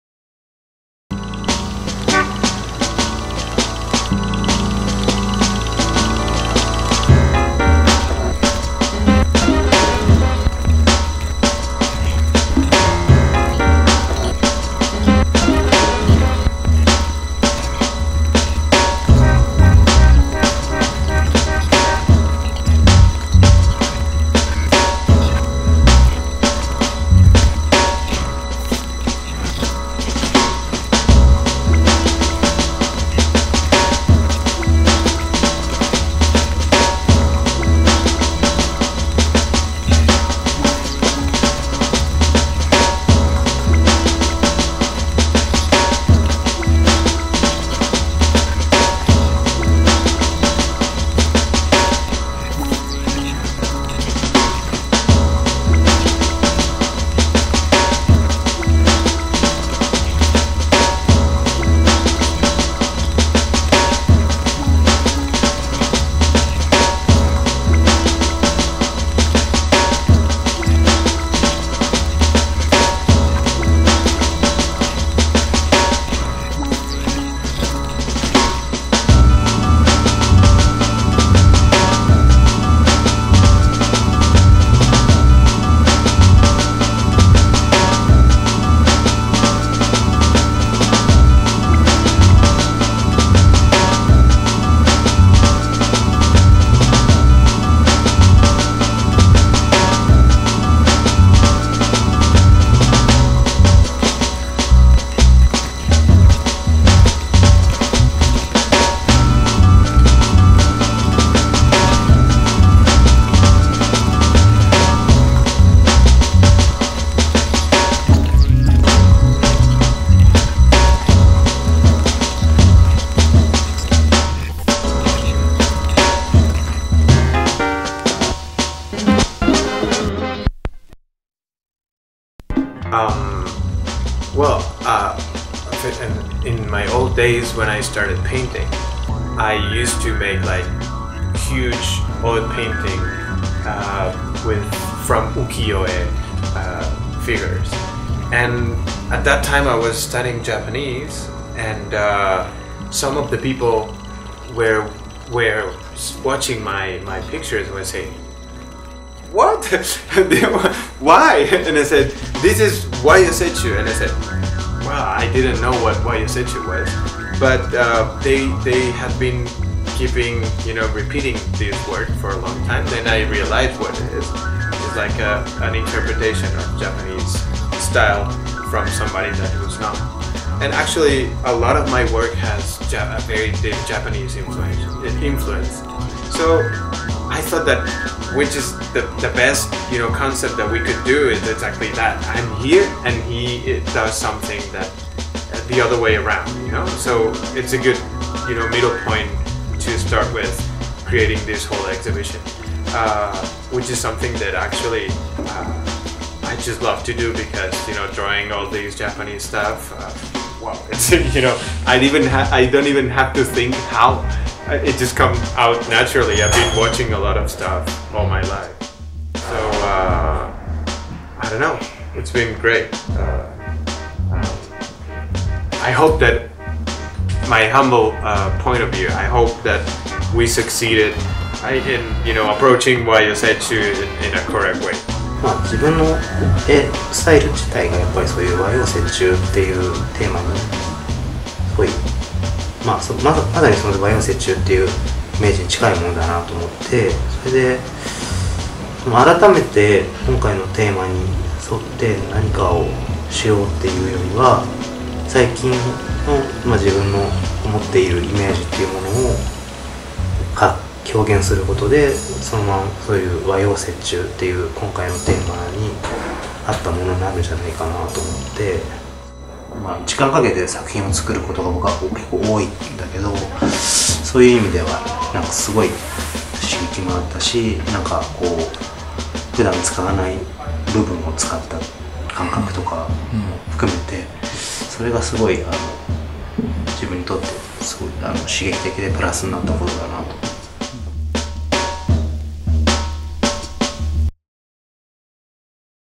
Wayo-Secchu by Aleix Gordo and Tadaomi Shibuya
Visual artist from Barcelona known for his explosive creative expression Aleix Gordo has collaborated with one of the most up and coming Japanese artist, Tadaomi Shibuya for a secret live painting event in Tokyo, on November 29th Friday. The event, titled Wayo-Secchu took place in Midori So, a latest addition to Tokyo's hidden creative spots, in front of selected audience. Wayo-Secchu is a Japanese term, which translates as a unique hybrid of Japanese style and Western style.

Aleix who grew up exposing to animation by Akira Toriyama or Katsuhiro Ohtomo greatly absorbed influences from these Japanese Manga culture. On the other hand, Tadaomi Shibuya, recognized in the international art scene, is famous for his trademark on radical illustration that renders images into stylized, angular abstractions of forms. This secret event was presented by JKD Collective, a Tokyo based creative boutique / production team which is known for its craft and experimental expression by Japan's leading innovators in music, visual design and technology.

バルセロナを拠点に、爆発的な表現力で注目を集めるヴィジュアルアーティスト、アレックス・ゴードが初来日。澁谷忠臣とのコラボレーションで、ライブペインティング、ミューラルアート制作を絡めたシークレット・パーティーが、11/29(金）、磁力を増す東京のクリエイティブスポット、みどり荘にて行われた。鳥山明や大友克典のアニメーションに幼少時からふれ、MANGAカルチャーの影響を大胆に取り込んできたアレックスに対し、様々なモチーフを直線的に再構築するスタイルが、欧米のシーンにおいて高い評価を得ている澁谷忠臣。興味深いバランスの上に立つ２名のアーティストによる和洋折衷のコラボレーションは、非常に刺激的でフレッシュなものになった。

Live Painting + Mural Art Prodction : Aleix Gordo, Tadaomi Shibuya
Produces : Bruce Ikeda /JKD Collective, Tomoji Oya, Miho Koshiba / midori.so
Camera : Hiro Ikematsu, Osamu Yotsya
Editer : 428